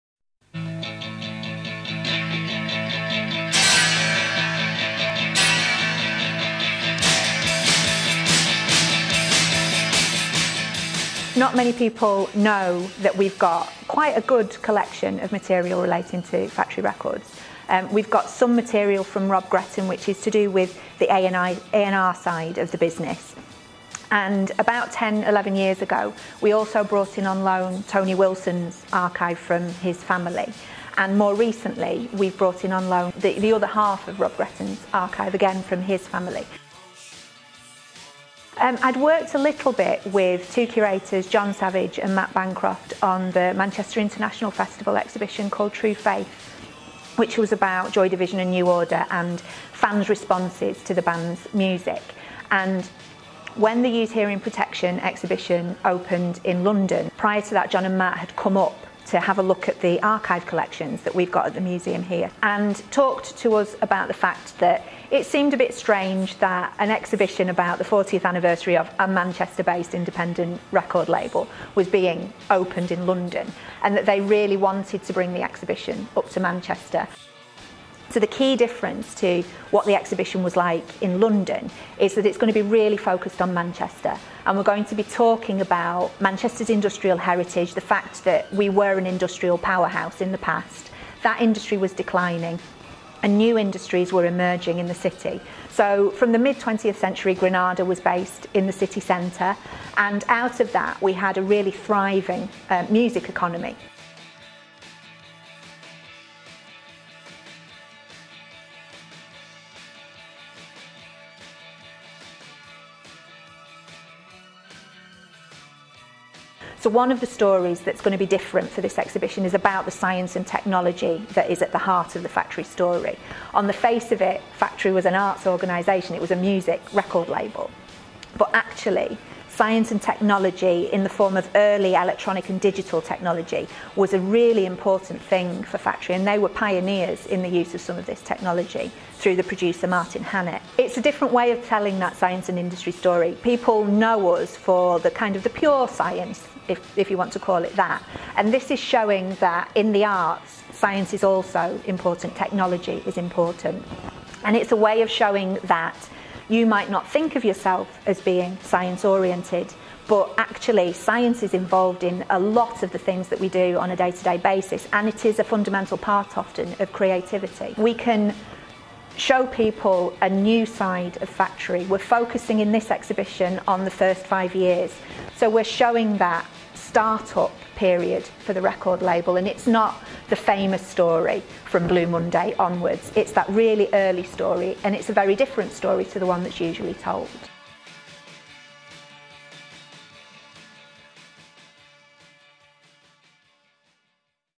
Use Hearing Protection at Science and Industry Museum, Manchester - the early Factory records story
Use Hearing Protection is an exhibition at the Science and Industry Museum, Manchester, which will tell the early story of Factory Records.
It includes artwork and other artefacts from the archives of Tony Wilson and Rob Gretton.
The exhibition runs from 19 June 2021 to 3 January 2022.
Talking about the exhibition in this interview with Marketing Manchester is curator Jan Hicks.